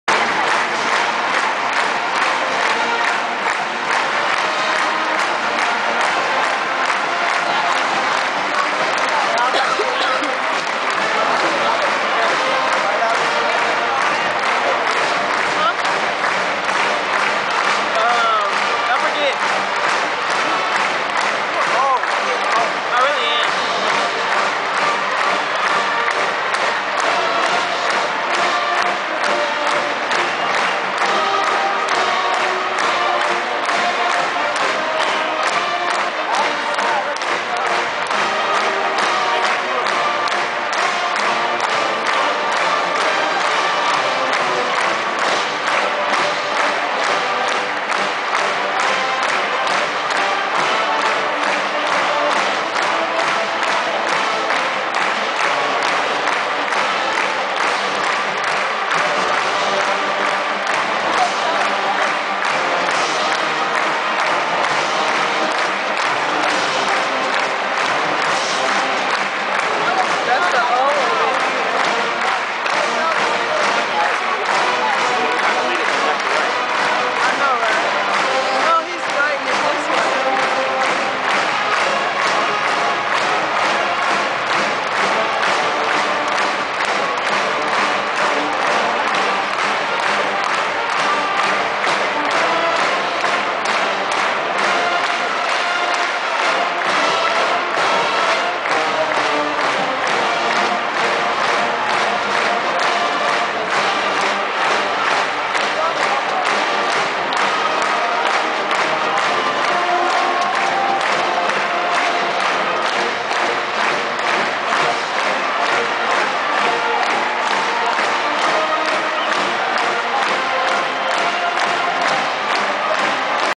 Ohio State Game "Script OHIO!"
i love when oue band does this!